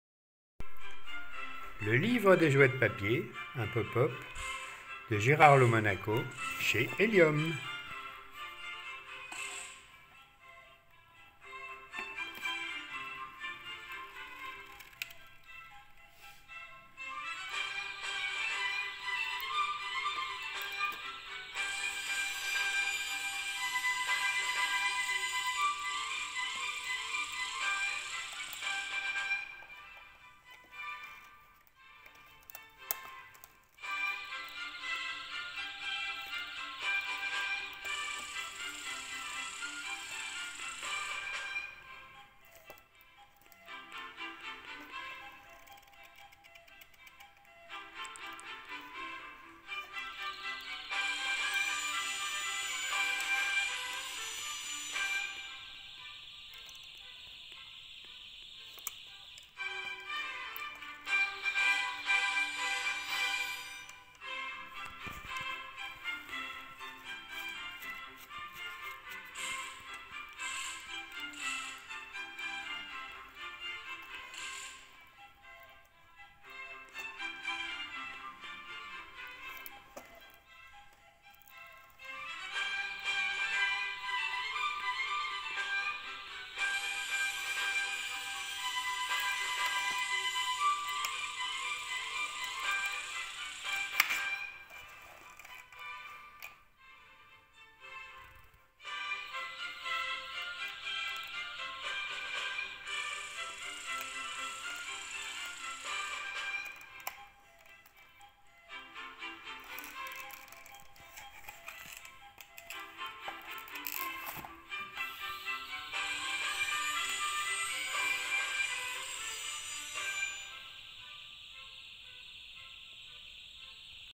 Le livre des jouets de papier Gérard Lo Monaco présenté par Pop-Up-Féerie
Gérard Lo Monaco nous fait retourner en enfance ! Simple, mais efficace